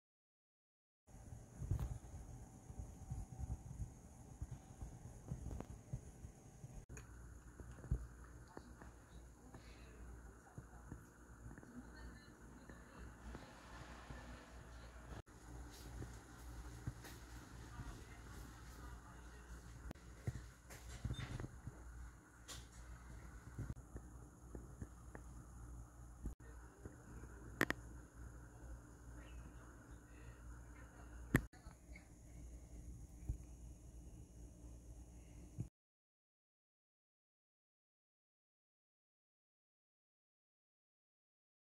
[1분 영상] 노을멜론 - 함안에서 온 국내 개발 신품종 멜론 리뷰: 넘치는 과즙, 부드러운 식감과 좋은 향기, 아름다운 녹색 과피를 갖춘 멜론.
경상남도 함안에서 개발된 노을멜론은 최근 급부상하고 있는 신품종 멜론입니다. 아름다운 초록색 과피와 오렌지색 과육이 인상적인 노을 멜론을 먹어보았는데요. 부드러운 식감의 과육과 넘치는 과즙, 그리고 아름다운 멜론향이 제대로 느껴지는 품종이었습니다.

껍질이 상대적으로 두껍다는 것을 제외하고는 별로 흠을 잡을 것이 없는 품종입니다. 최근 계속적으로 맛있는 품종들이 등장하는 가운데 한국 멜론의 거듭되는 발전을 기대해봅니다.